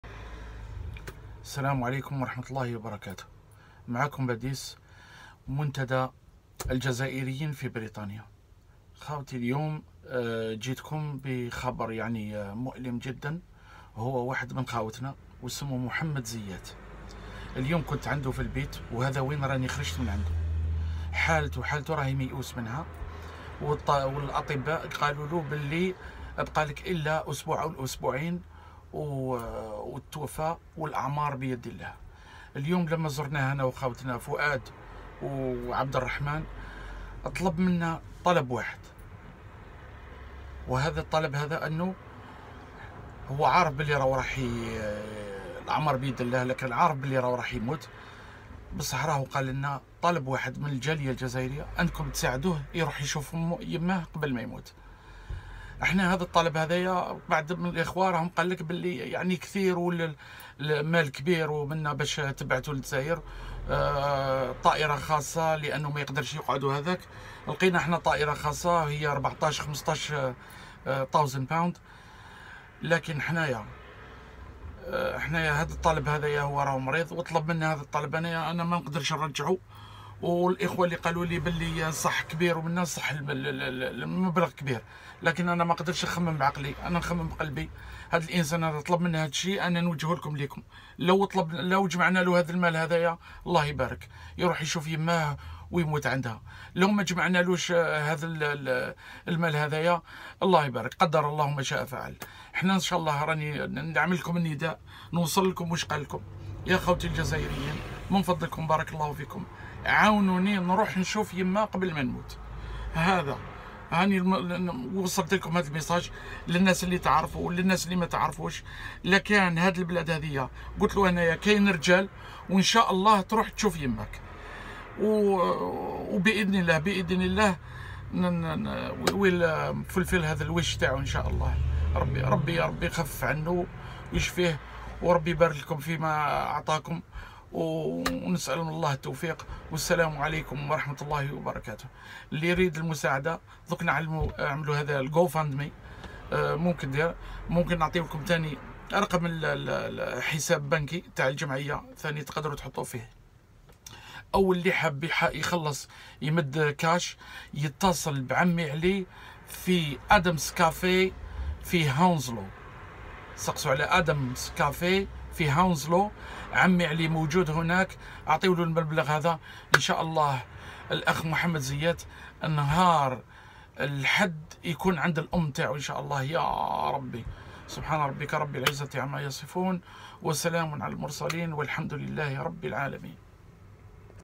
Complain to raise £15000 for Mohamed ziat
We need £15000 to pay for private plane